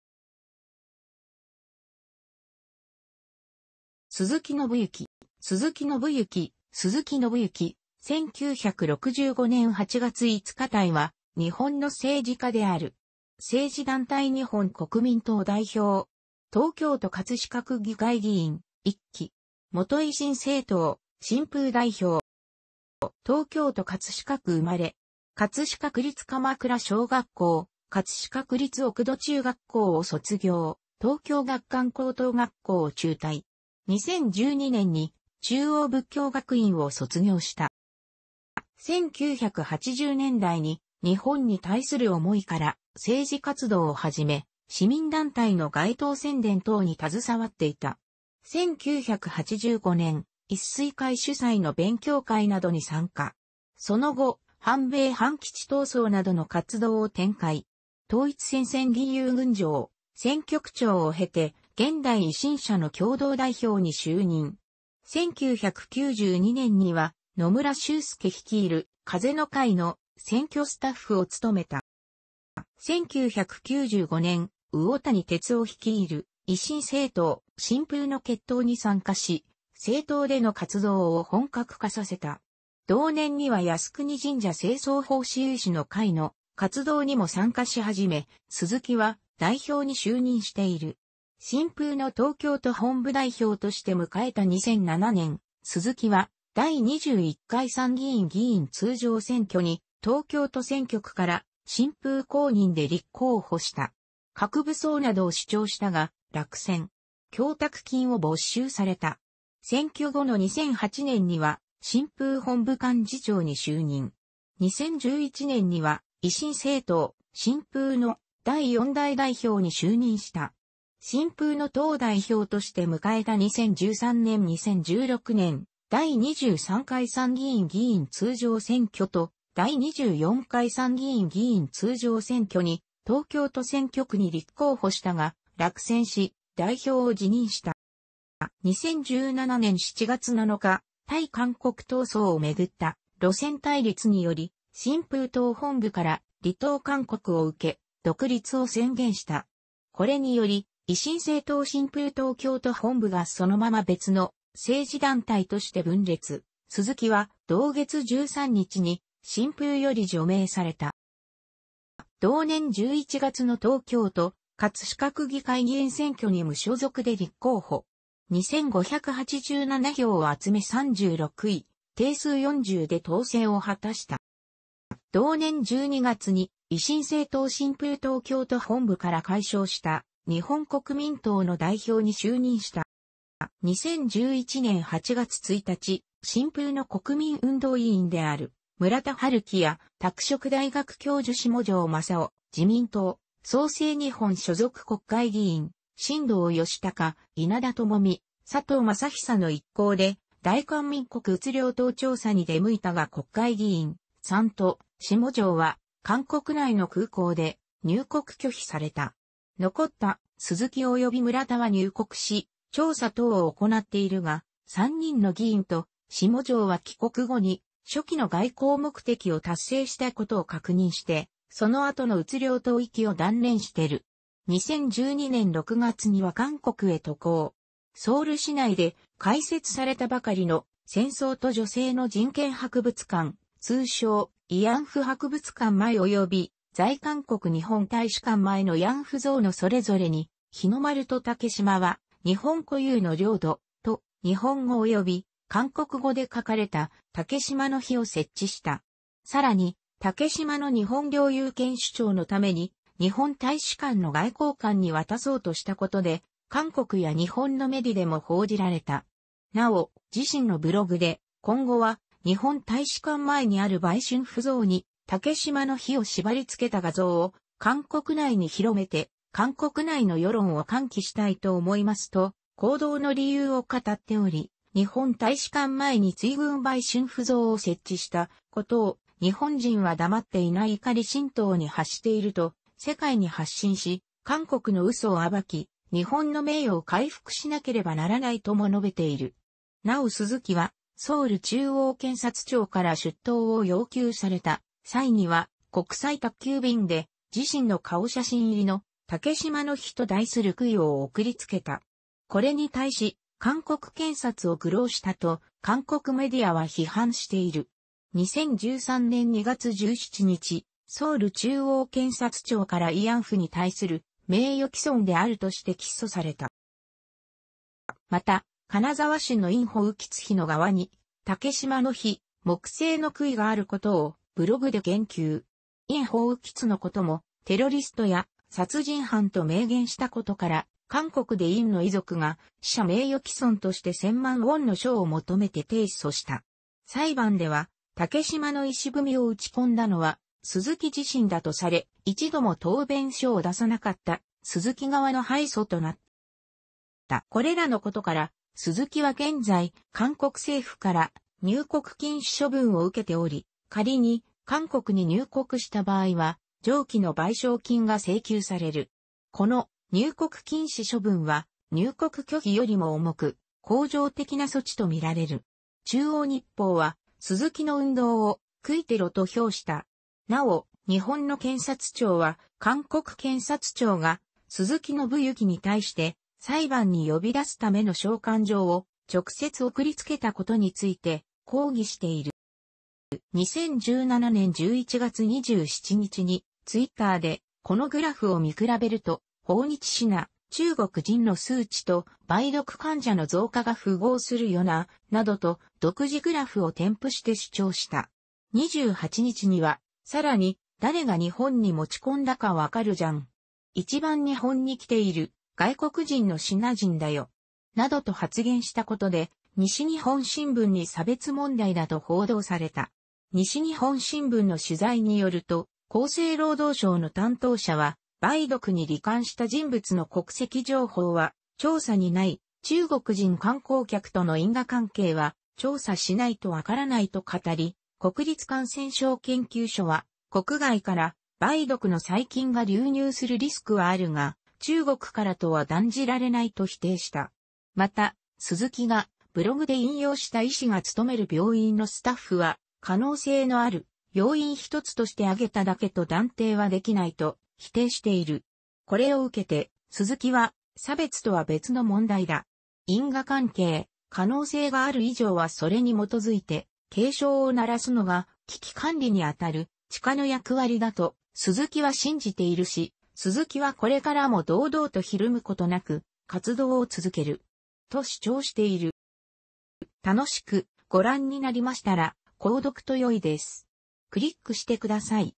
鈴木信行
鈴木 信行（すずき のぶゆき、1965年8月5日 - ）は、日本の政治家である。政治団体日本国民党代表。東京都葛飾区議会議員（1期）。元維新政党・新風代表。

東京都葛飾区生まれ。葛飾区立鎌倉小学校、葛飾区立奥戸中学校を卒業、東京学館高等学校を中退。2012年に中央仏教学院を卒業した。

1980年代に日本に対する思いから政治活動を始め、市民団体の街頭宣伝等に携わっていた。1985年、一水会主催の勉強会などに参加。その後、反米反基地闘争などの活動を展開。統一戦線義勇軍情宣局長を経て、現代維新社の共同代表に就任。1992年には、野村秋介率いる「風の会」の選挙スタッフを務めた。

1995年、魚谷哲央率いる維新政党・新風の結党に参加し、政党での活動を本格化させた。同年には靖国神社清掃奉仕有志の会の活動にも参加し始め、鈴木は代表に就任している。新風の東京都本部代表として迎えた2007年、鈴木は第21回参議院議員通常選挙に東京都選挙区から新風公認で立候補した。核武装などを主張したが、落選。供託金を没収された。選挙後の2008年には新風本部幹事長に就任。2011年には維新政党・新風の第4代代表に就任した。新風の党代表として迎えた2013年・2016年、第23回参議院議員通常選挙と第24回参議院議員通常選挙に東京都選挙区に立候補したが、落選し、代表を辞任した。

2017年7月7日、対韓国闘争を巡った路線対立により、新風党本部から離党勧告を受け、独立を宣言した。これにより、「維新政党新風東京都本部」がそのまま別の政治団体として分裂、鈴木は同月13日に新風より除名された。

同年11月の東京都葛飾区議会議員選挙に無所属で立候補。2587票を集め36位（定数40）で当選を果たした。

同年12月に「維新政党新風東京都本部」から改称した「日本国民党」の代表に就任した。。

2011年8月1日、新風の国民運動委員である村田春樹や拓殖大学教授下條正男、自民党・創生日本所属国会議員新藤義孝、稲田朋美、佐藤正久の一行で大韓民国鬱陵島調査に出向いたが国会議員三名と下條は韓国内の空港で入国拒否された。残った鈴木及び村田は入国し調査等を行っているが、三人の議員と下條は帰国後に所期の外交目的を達成した事を確認してその後の鬱陵島行きを断念している。
2012年6月には韓国へ渡航。ソウル市内で、開設されたばかりの「戦争と女性の人権博物館」（通称・慰安婦博物館）前及び在韓国日本大使館前の慰安婦像のそれぞれに、日の丸と"竹島は日本固有の領土"と日本語および韓国語で書かれた「竹島の碑」を設置した。さらに、竹島の日本領有権主張の為に日本大使館の外交官に渡そうとしたことで韓国や日本のメディアでも報じられた。なお、自身のブログで『今後は日本大使館前にある売春婦像に「竹島の碑」を縛りつけた画像を韓国内に広めて、韓国内の世論を喚起したいと思います』と、行動の理由を語っており、『日本大使館前に追軍売春婦像を設置したことを、日本人は黙っていない怒り心頭に発していると、世界に発信し韓国の嘘を暴き、日本の名誉を回復しなければならない』とも述べている。なお鈴木はソウル中央検察庁から出頭を要求された際には国際宅急便で自身の顔写真入りの「竹島の碑」と題する杭を送り付けた。これに対し「韓国検察を愚弄した」と韓国メディアは批判している。2013年2月17日、ソウル中央検察庁から慰安婦に対する名誉毀損であるとして起訴された。
また金沢市の尹奉吉碑の側に「竹島の碑」（木製の杭）があることをブログで言及。尹奉吉のこともテロリストや殺人犯と明言したことから、韓国で尹の遺族が死者名誉毀損として1千万ウォンの賠償を求めて提訴した。裁判では竹島の碑を打ち込んだのは鈴木自身だとされ、一度も答弁書を出さなかった鈴木側の敗訴となった。
これらのことから鈴木は現在、韓国政府から入国禁止処分を受けており、仮に韓国に入国した場合は上記の賠償金が請求される。この「入国禁止」処分は「入国拒否」よりも重く、恒常的な措置とみられる。
中央日報は、鈴木の運動を「杭テロ」と評した。
なお、日本の検察庁は、韓国検察庁が鈴木信行に対して裁判に呼び出すための召喚状を直接送りつけたことについて、抗議している。

2017年11月27日に、twitterで「このグラフを見比べると、訪日支那（中国）人の数値と梅毒患者の...